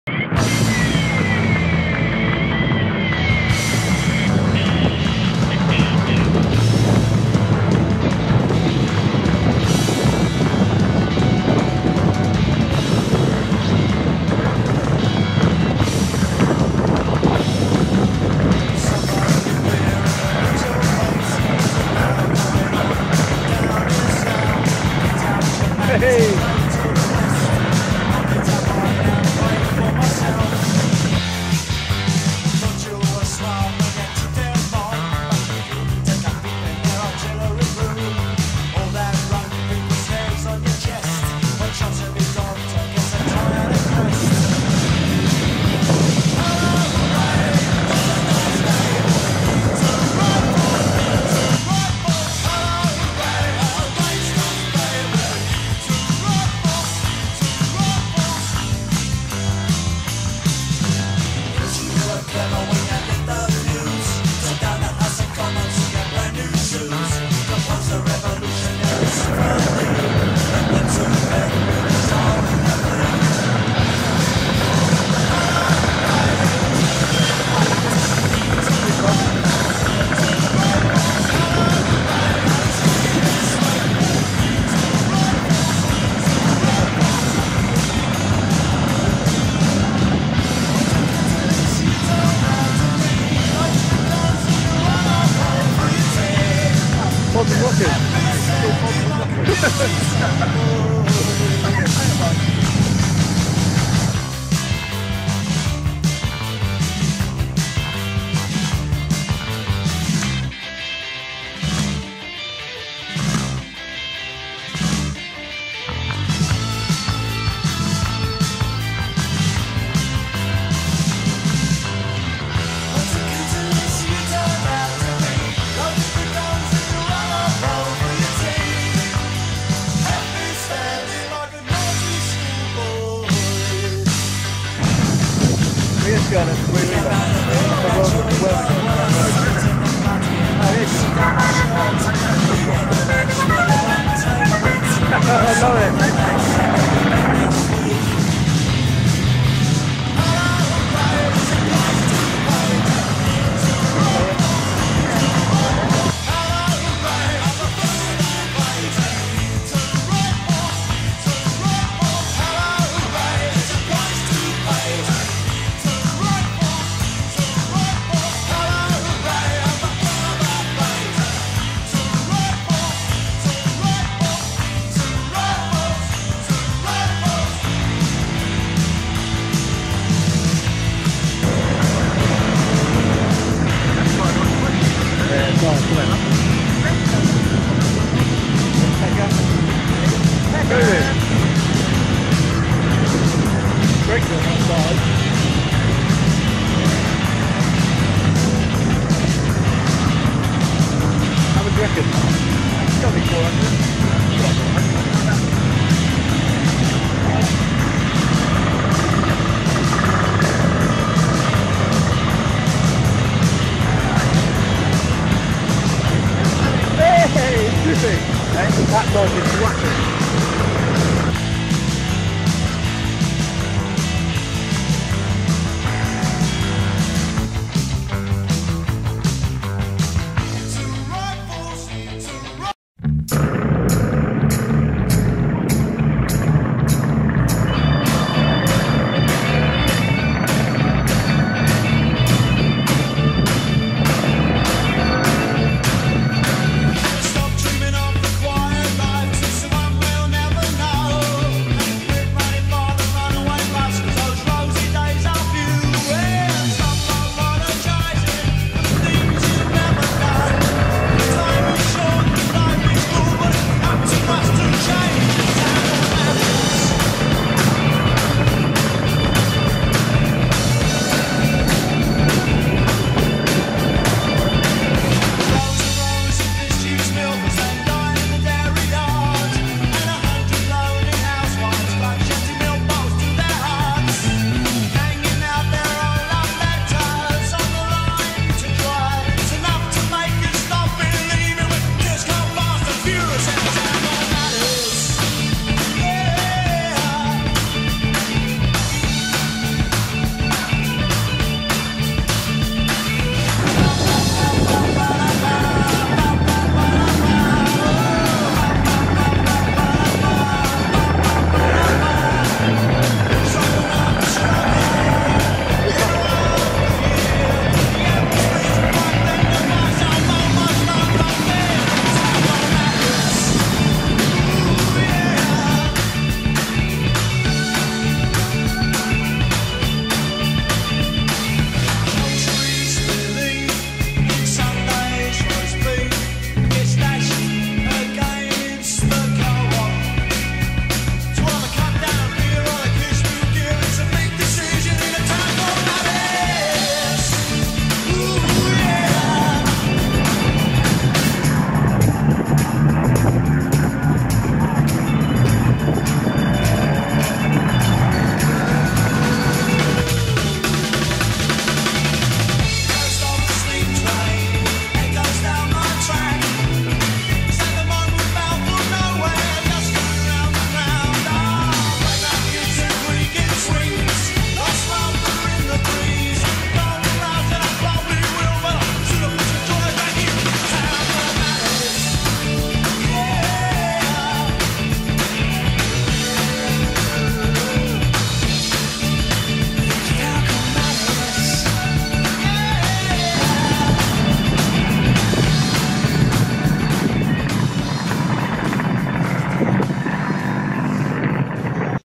Mods and Rockers 2011 Isle of Wight
Mods and Rockers Round the Island Run 2011 for the Earl Mountbatten Hospice Isle of Wight.